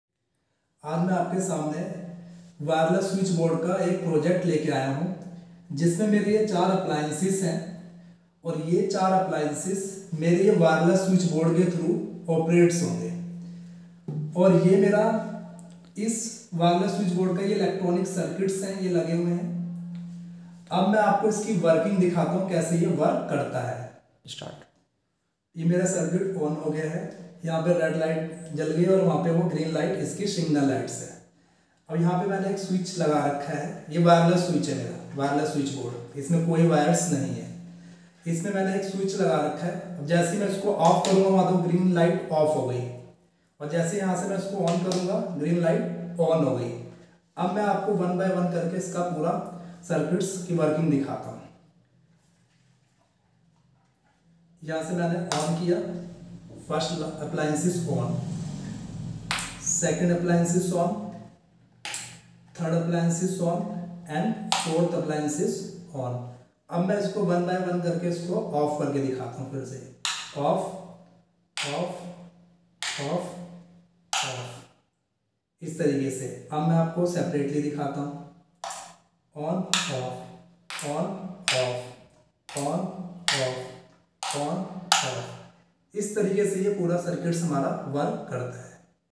Wireless Switch Board
A wireless switch board is a type of control system that allows you to remotely turn electrical devices on and off. Instead of using traditional wired switches, a wireless switch board uses radio frequency (RF) signals to communicate with the devices. This allows you to control lights, fans, appliances, and other electrical equipment from a remote location, typically through a smartphone app or a dedicated remote control.
Wireless switch boards can be particularly useful in situations where it is difficult or impossible to install traditional wired switches, such as in historic buildings, rental properties, or where access to the electrical wiring is limited. They can also provide a convenient way to control lighting and other appliances from the comfort of your couch or bed.
In addition to remote control, some wireless switch boards also offer features like scheduling, energy monitoring, and integration with smart home systems. Overall, wireless switch boards offer a flexible, convenient, and cost-effective solution for controlling electrical devices in your home or business.